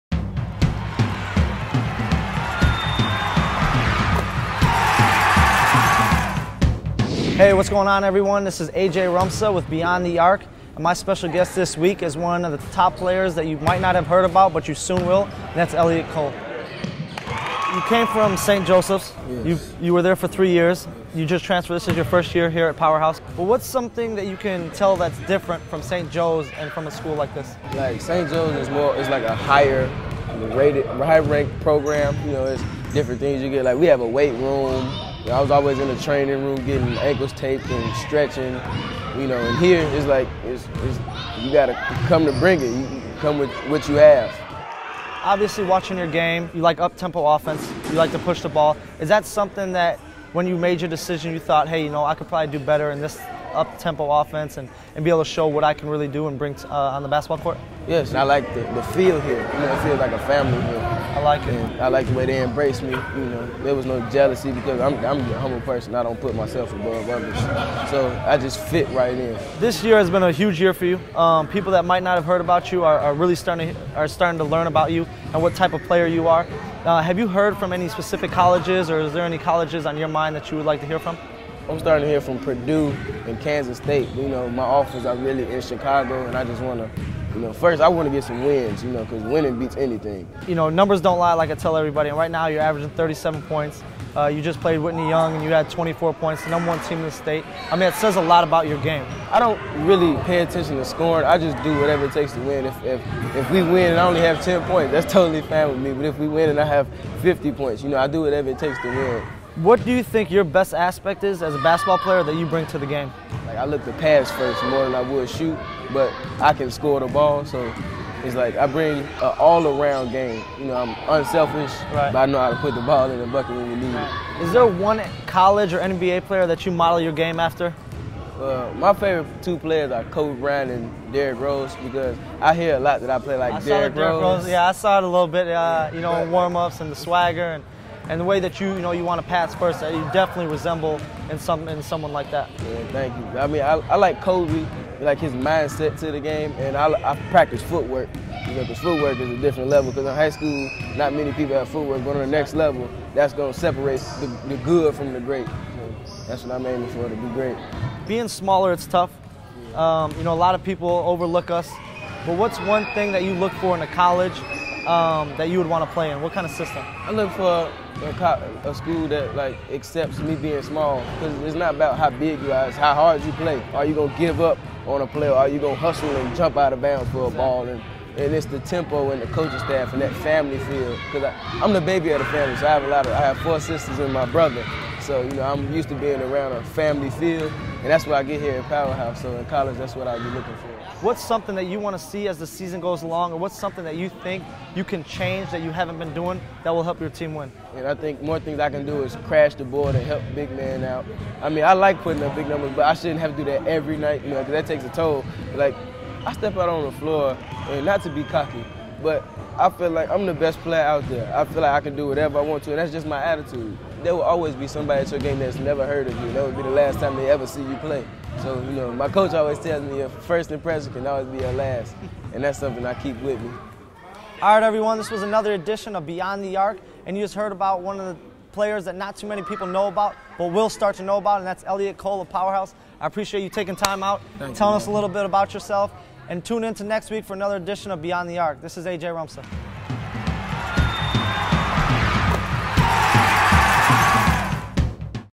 MVP: Elliott Cole
AJ Rompza sits down with six-foot Power House senior guard Elliott Cole after a game versus Whitney Young. Cole talks about his tranfser from St. Joseph, working as a team to get the win, and his future in college basketball.